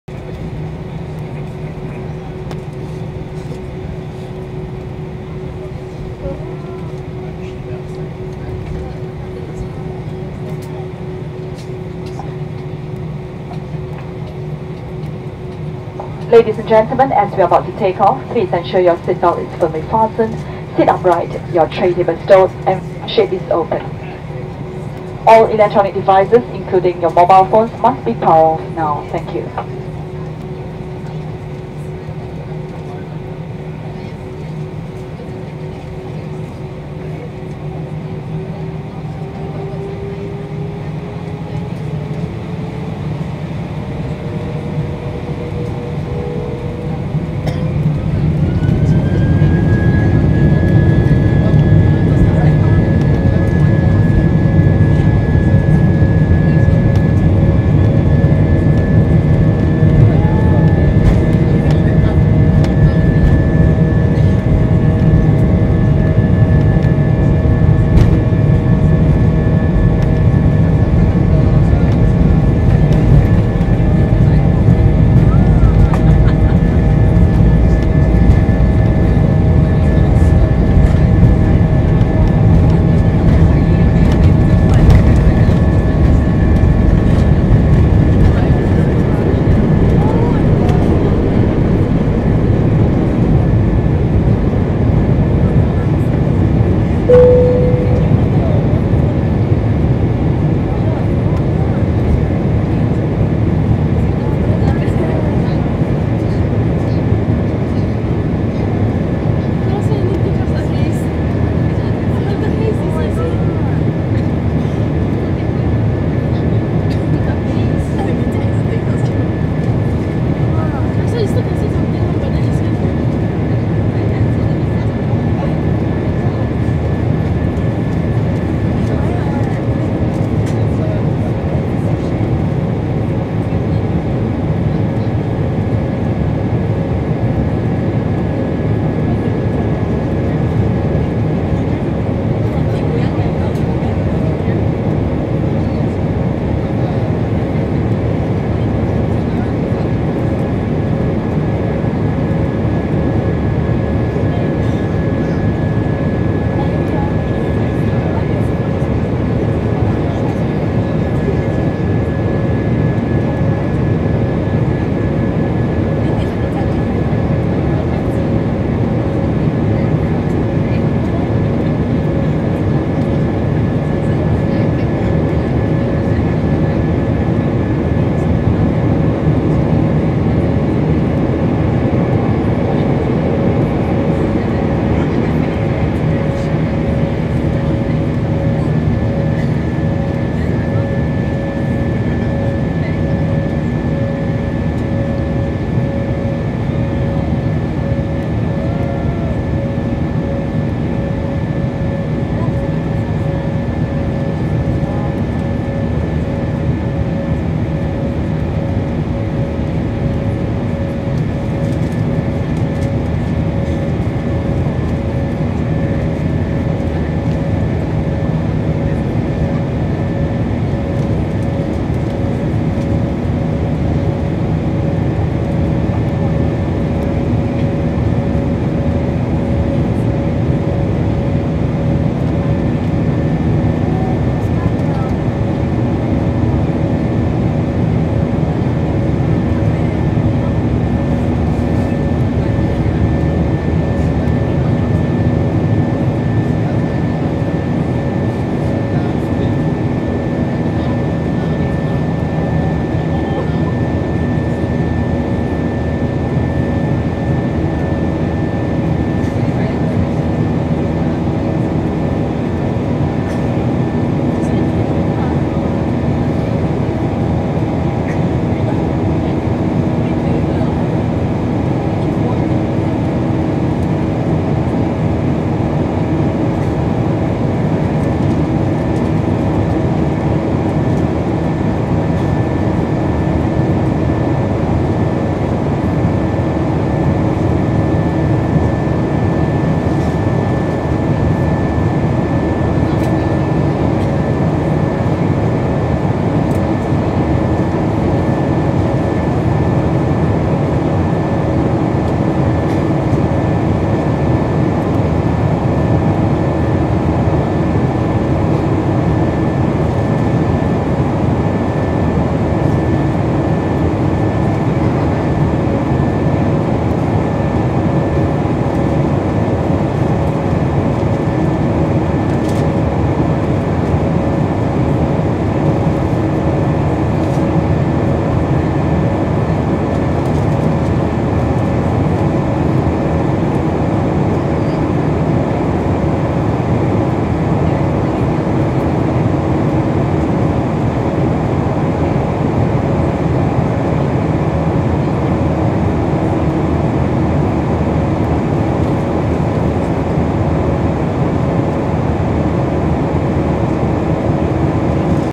JetStar Asia Airbus A320 Takeoff - Singapore (3K 535)
Takeoff in Singapore on board JetStar Asia. The visibility was very bad due to the haze.

Airline - JetStar Asia (3K/JSA)
Aircraft - Airbus A320-200
Airport - Singapore Changi Airport (SIN/WSSS)
Runway - 20C
Flight - 3K 535
To - Phuket International Airport (HKT/VTSP)
Registration - 9V-JSH ("SG 50" livery)
Seat - 10F
Date - 13/09/15